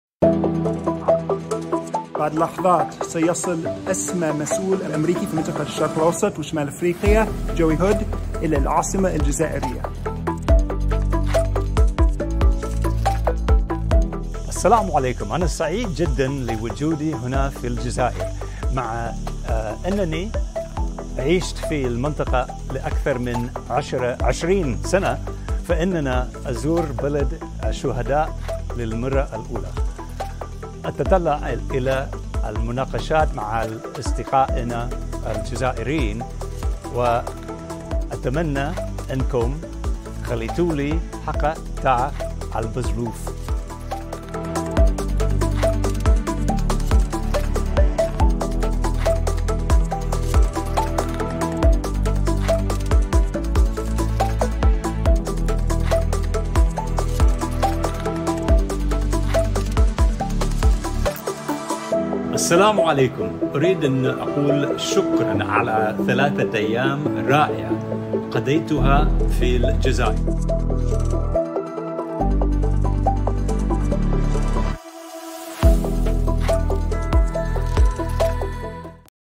Ce réprésentant américain veut du Bouzelouf ! 😂🐏🍴
Joey Hood, le secrétaire d'Etat adjoint américain aux affaires du Proche-Orient demande aux algériens de lui laisser une part de Bouzelouf ! 😂🐏🍴